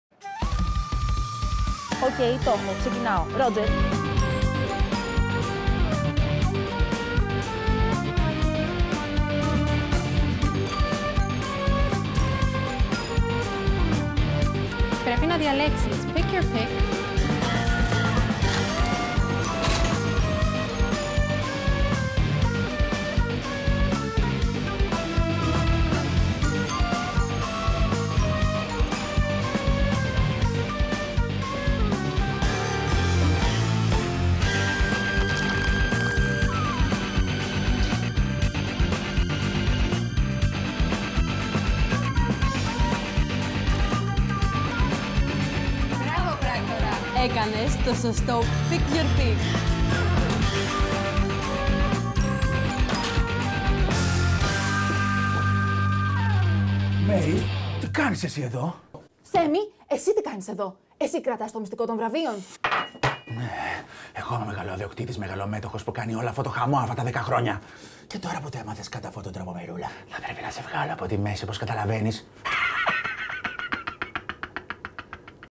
Ποιός κρύβει το μυστικό των Mad VMA? (short movie by Pick Crackers)
Δες την "ταινία μικρού μήκους" με πρωταγωνιστές τους Μαίρη Συνατσάκη και Θέμη Γεωργαντά!

Ποιός κρύβει το μυστικό των βραβείων?

By Pick Crackers Papadopoulou!